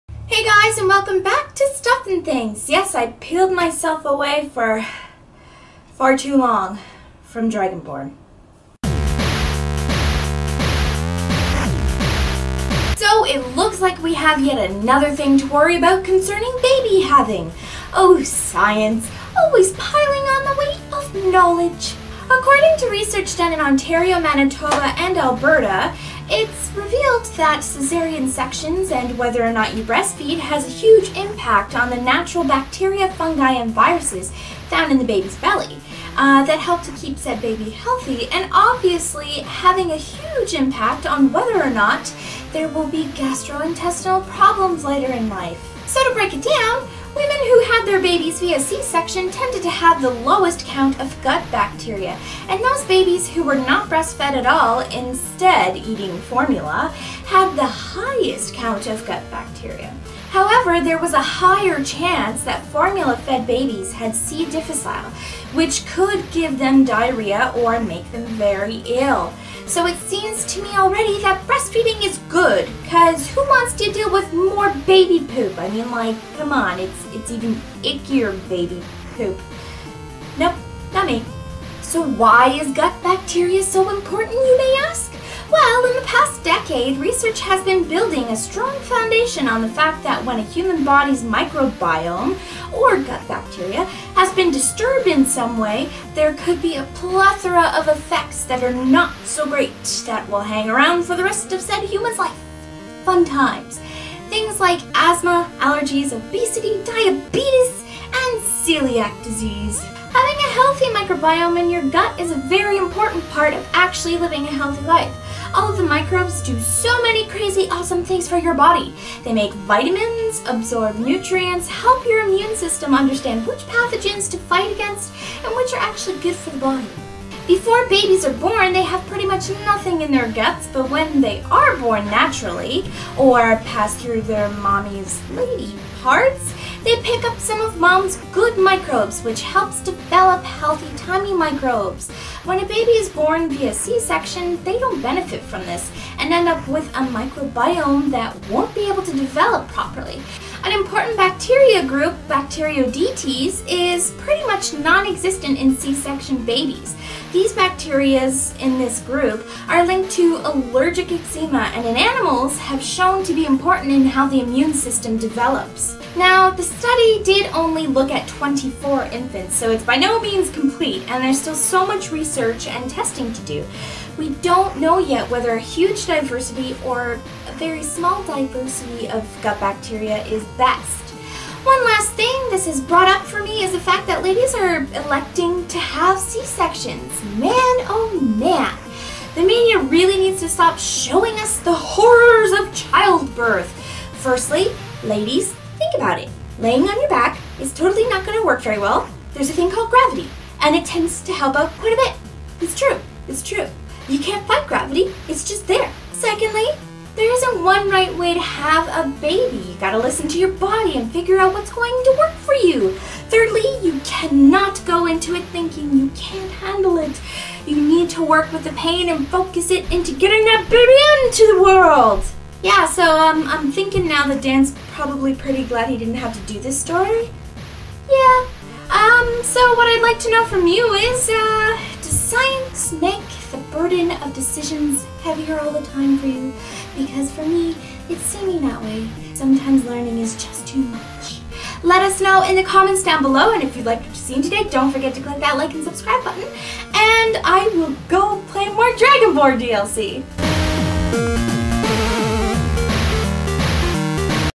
Why You Should Be a Natural Mamma!
Mia talks about what scientists have found out about the differences between c-sections and natural births, and breastfeeding and formula feeding.

T-shirts!

Follow Us on Tweeter And Never Miss An Episode!

Face in your Book!